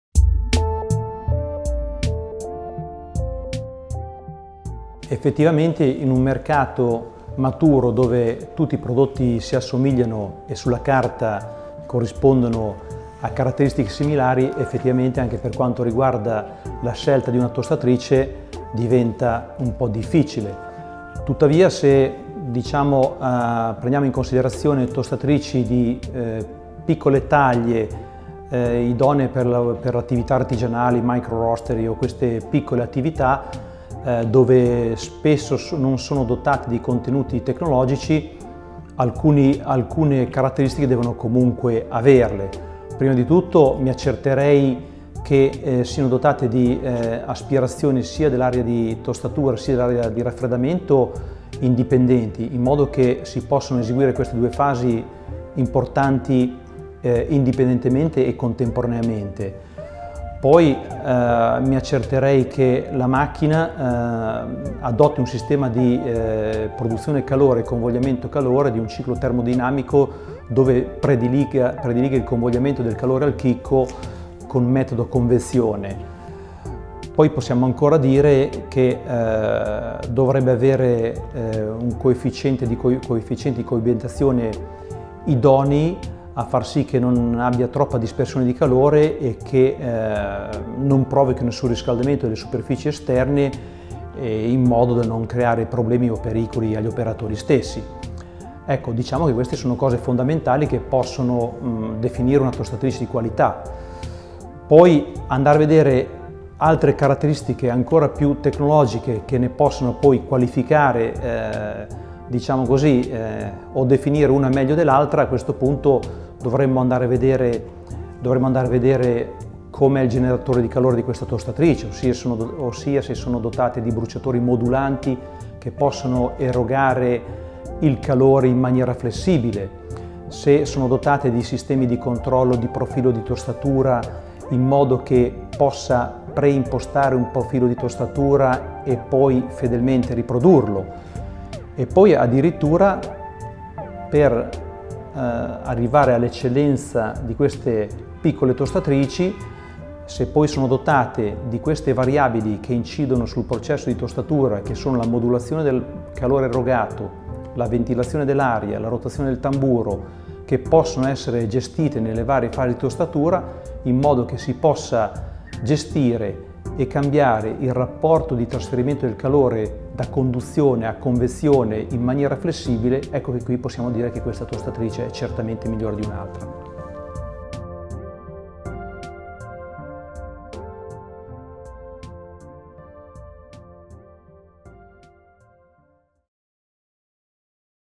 Trieste Coffee Experts 2017: Roberto Pedini "Come scegliere una tostatrice"
Roberto Pedini (Sales Product Development Manager IMA PETRONCINI) ci spiega come scegliere la migliore tostatrice per il caffè in base alle diverse esigenze produttive: possiamo identificare una macchina per piccole quantità idonea ad una realtà artigianale e di micro roastery con contenuti tecnologici ridotti, oppure orientarci su una tostatrice idonea a produrre ingenti quantità di caffè tostato dotata di tecnologie elevate e parametri più specifici.